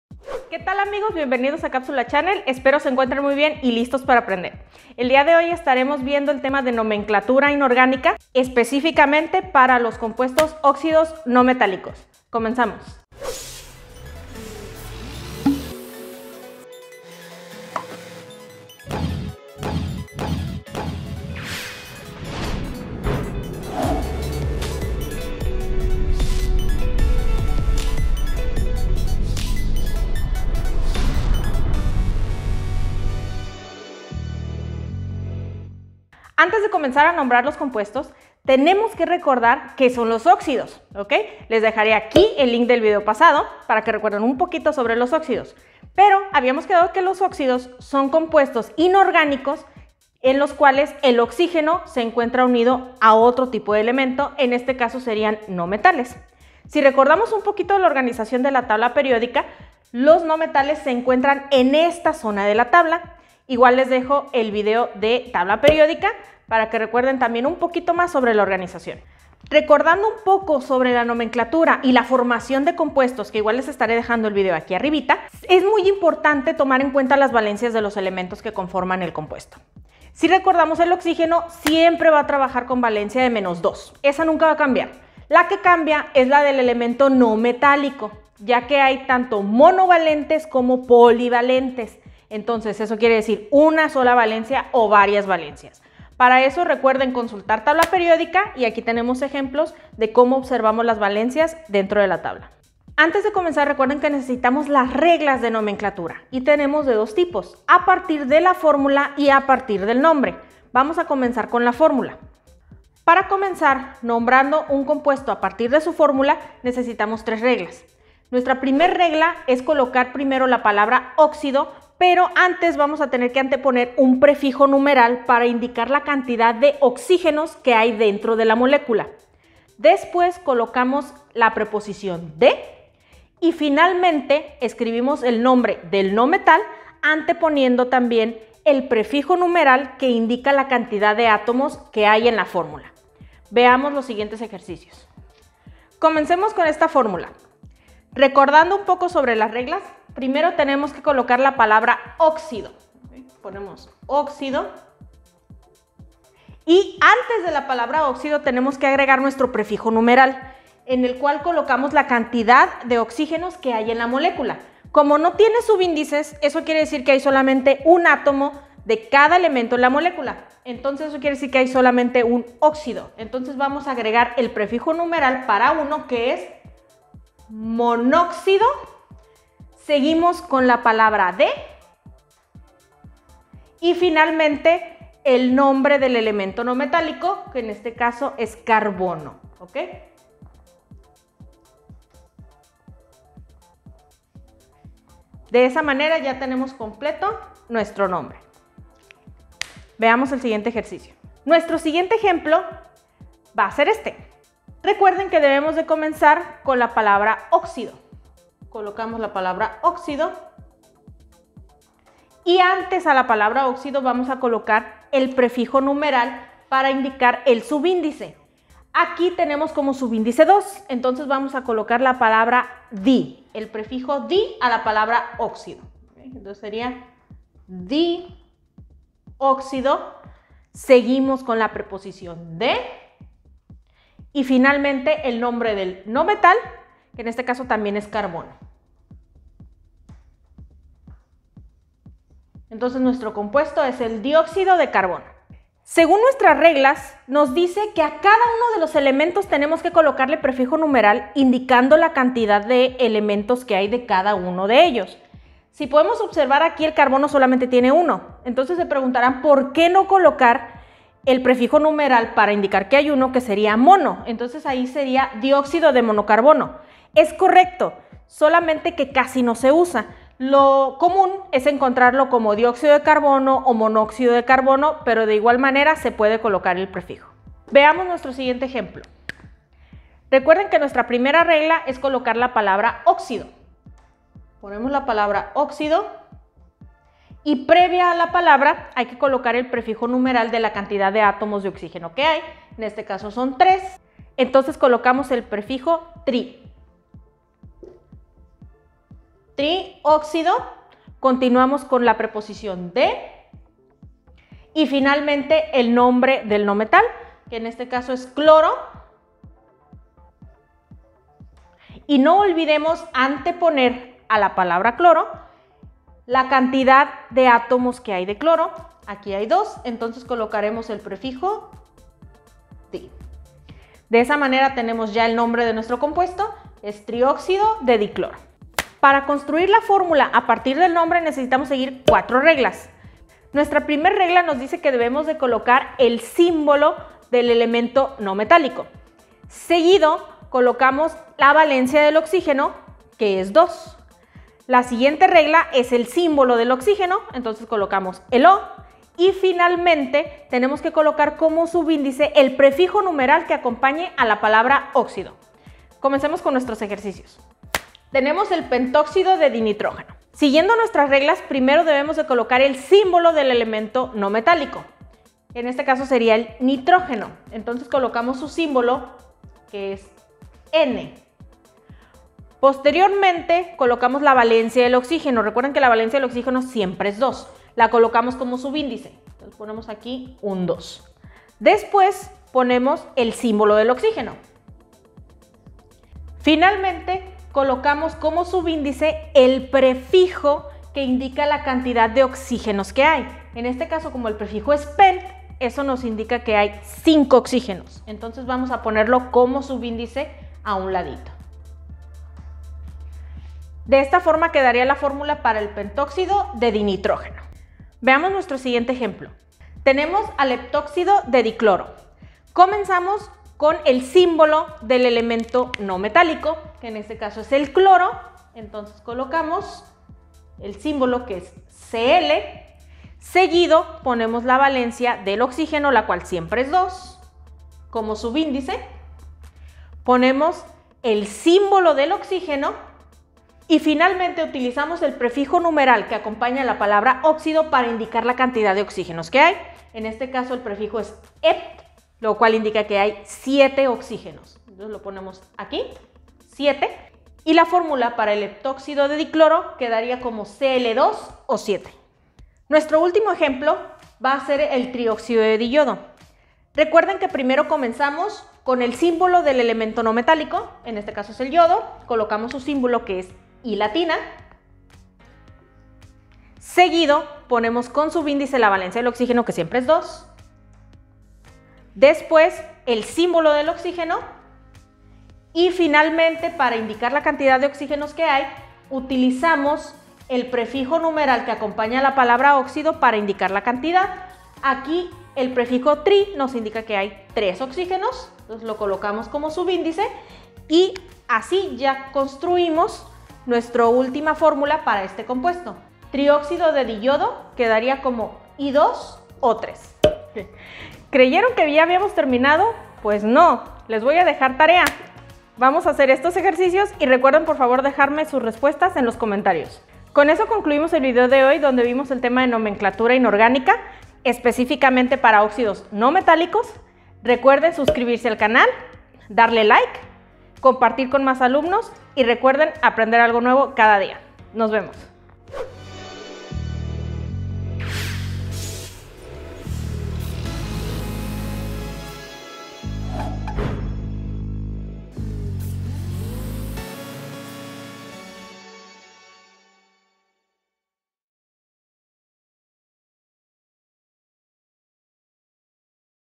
Nomenglatura de Óxidos No Metálicos /Tutorial //Fácil y Sencillo🔥🔥🚀
Con este tutorial serás un Crack en Nomenglatura de compuestos inorgánicos, especialmente de los óxidos no metálicos y claro siempre de una forma fácil y sencilla.

Saludos